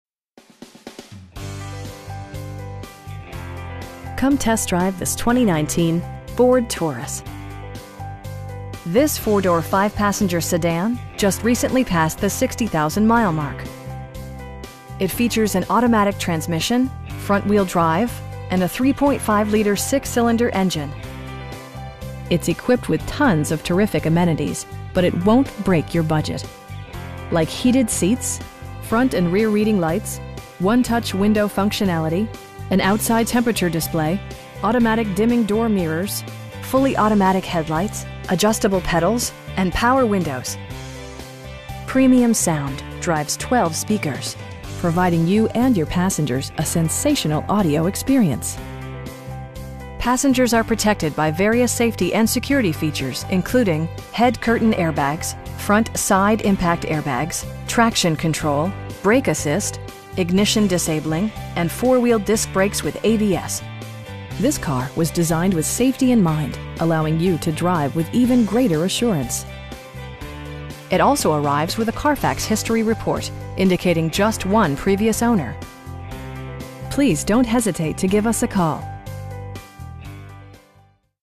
2019 Ford Taurus Limited in Overland Park, KS 66212
Bob Allen Ford
9239 Metcalf Avenue

Come test drive this 2019 Ford Taurus. This 4 door, 5 passenger sedan just recently passed the 60,000 mile mark! It features an automatic transmission, front-wheel drive, and a 3.5 liter 6 cylinder engine. It's equipped with tons of terrific amenities, but it won't break your budget. Like heated seats, front and rear reading lights, 1-touch window functionality, an outside temperature display, automatic dimming door mirrors, fully automatic headlights, adjustable pedals, and power windows. Premium sound drives 12 speakers, providing you and your passengers a sensational audio experience. Passengers are protected by various safety and security features, including: head curtain airbags, front side impact airbags, traction control, brake assist, ignition disabling, an emergency communication system, and 4 wheel disc brakes with ABS. This car was designed with safety in mind, allowing you to drive with even greater assurance. It also arrives with a Carfax history report, indicating just one previous owner. We pride ourselves on providing excellent customer service. Please don't hesitate to give us a call.